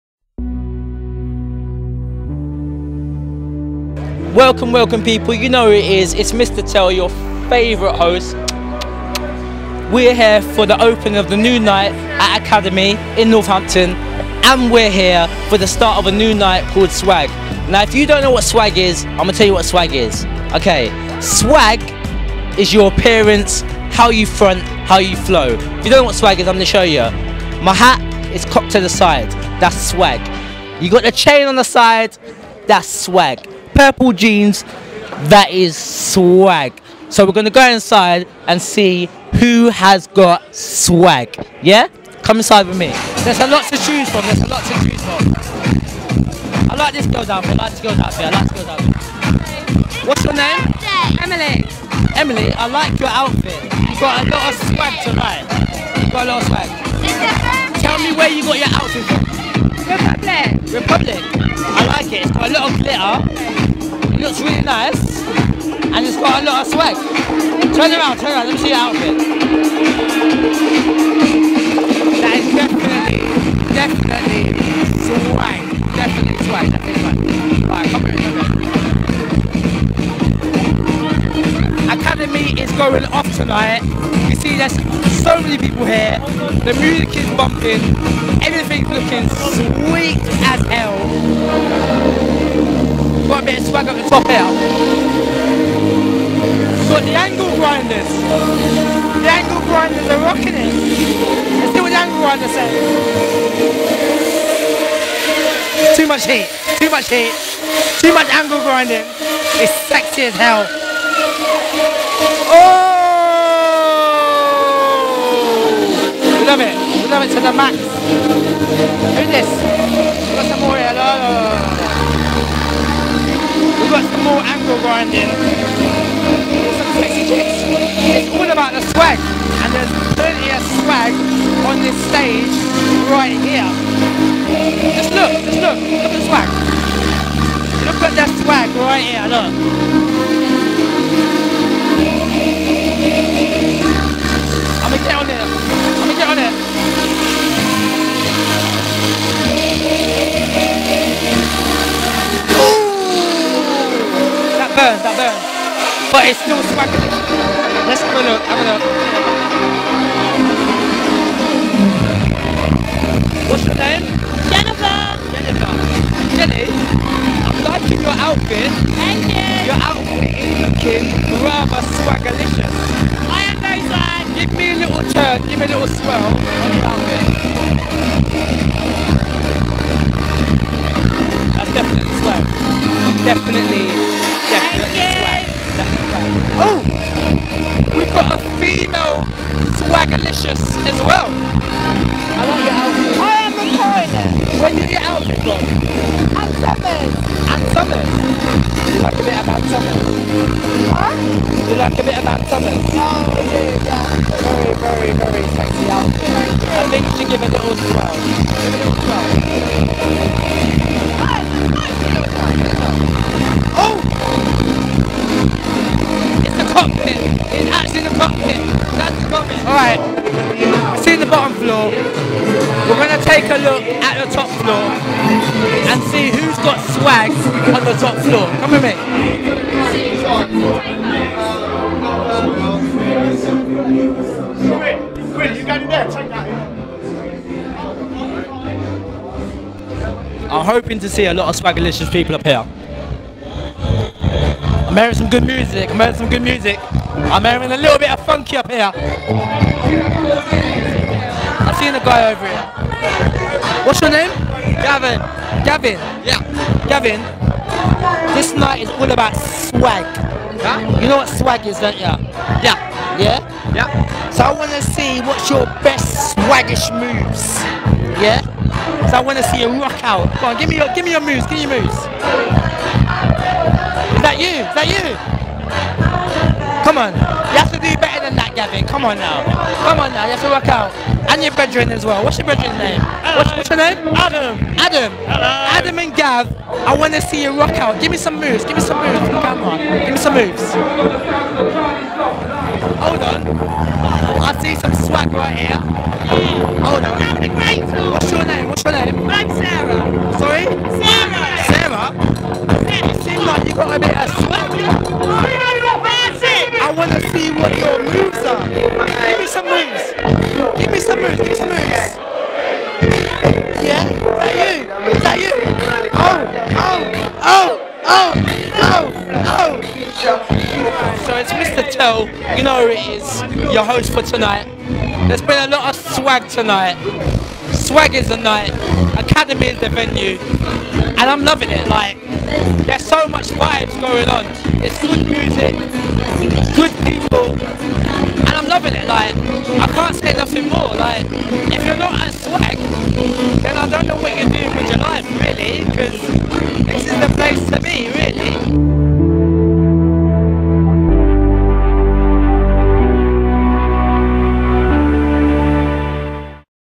Swag's Finding Swag @ Academy
Check out our very own Jamal as he looks for Swag at the opening night of the best Saturday night in Northampton.